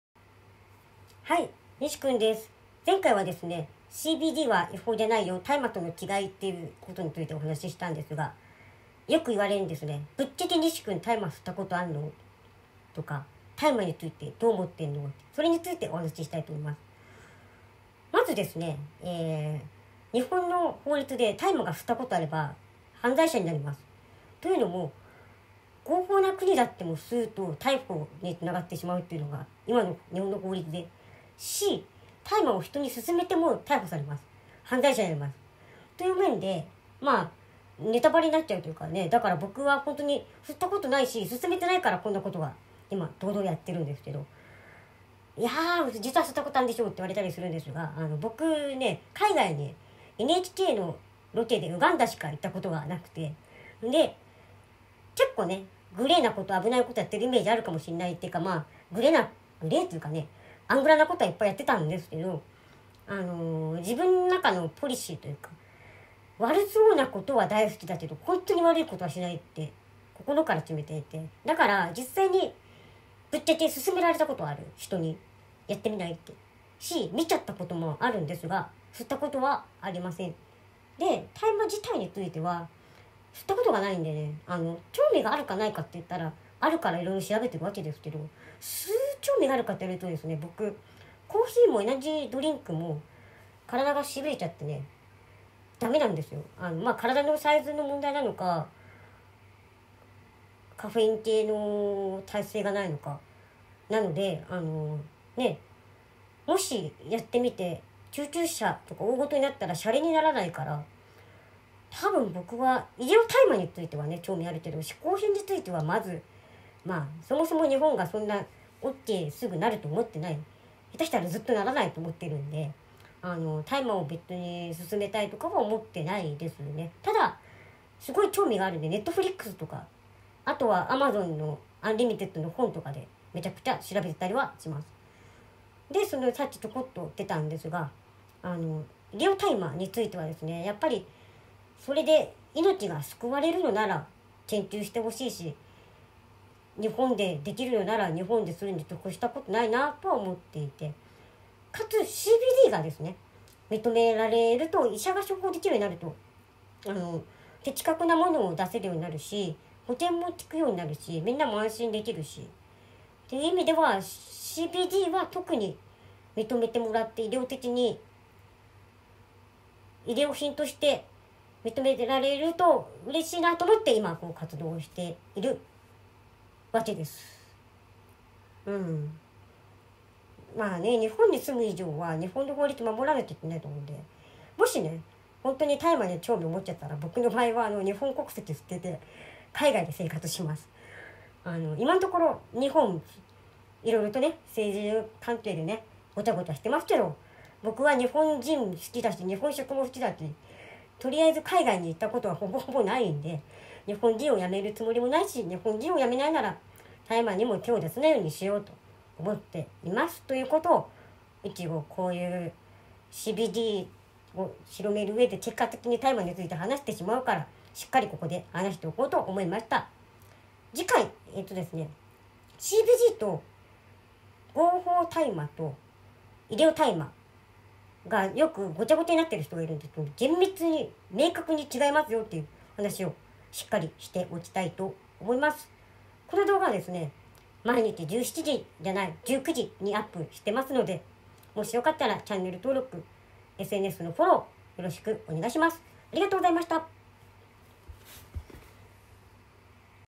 ぶっちゃけ吸ったことある？｜大麻についてKOHEY NISHIの考え
CBDについて伝える上で、どうしても大麻に関しての内容も増えている。ここで一度、正直な事実報告。大麻を吸っているか吸ったことあるか否かと、大麻についてKOHEY NISHI（にしくん）の考えをまとめておく。

この動画と類似内容の詳しい記事をブログに上げています。